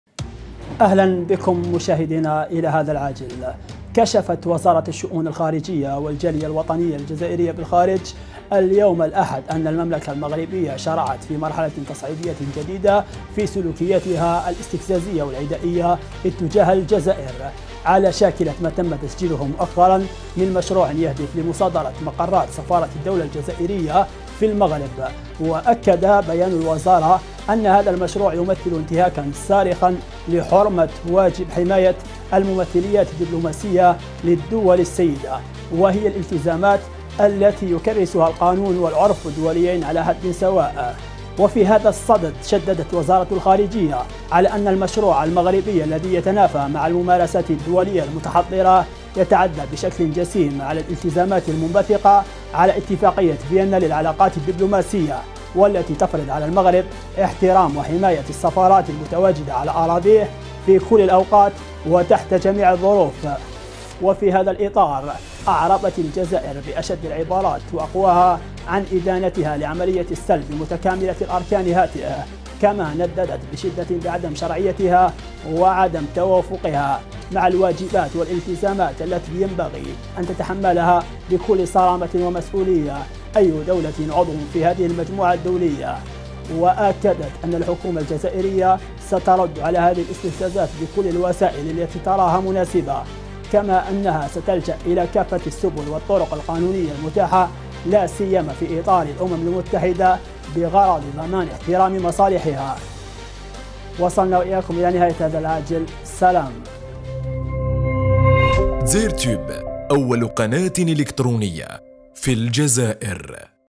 عاجل ..وزارة الخارجية و الجالية الوطنية بالخارج ترد على إستفزازات المخ.زن و الإنتهاك الصارخ
عاجل ..وزارة الخارجية و الجالية الوطنية بالخارج ترد على إستفزازات المخ.زن و الإنتهاك الصارخ لمقرات سفارة الدولة الجزائرية بالمغ.رب.. و تؤكد أنها سترد بإستعمال كافة السبل و الطرق القانونية المتاحة